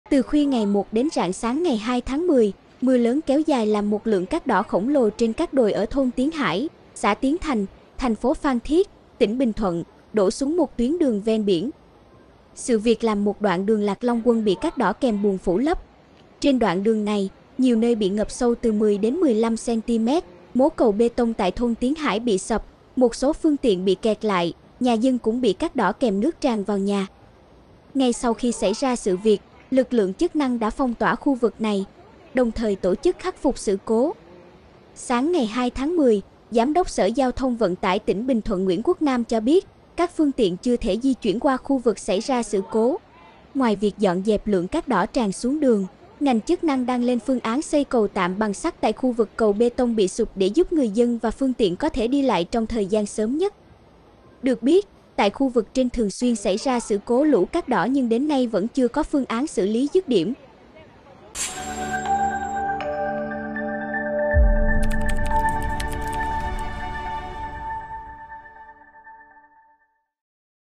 Lũ cát đỏ ập xuống đường ven biển TP Phan Thiết
Lũ cát đỏ làm một đoạn đường bị phủ lấp, nhiều nơi bị ngập sâu từ 10-15cm, mố cầu bê tông tại thôn Tiến Hải bị sập, một số phương tiện bị kẹt lại, nhà dân bị cát đỏ kèm nước tràn vào nhà.